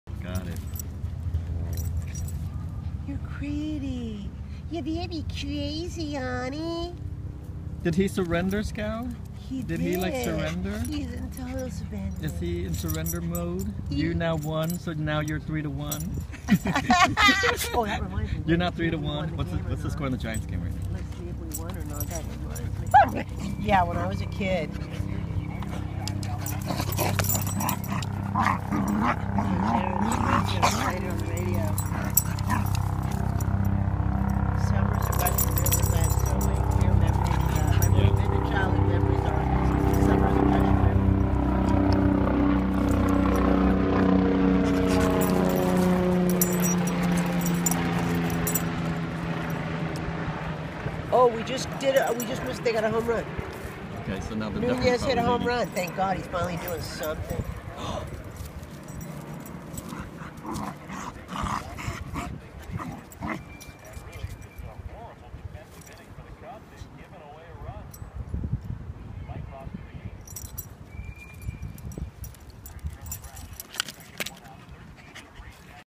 Beagle Scout takes the lead
Scout has a few good wrestling moves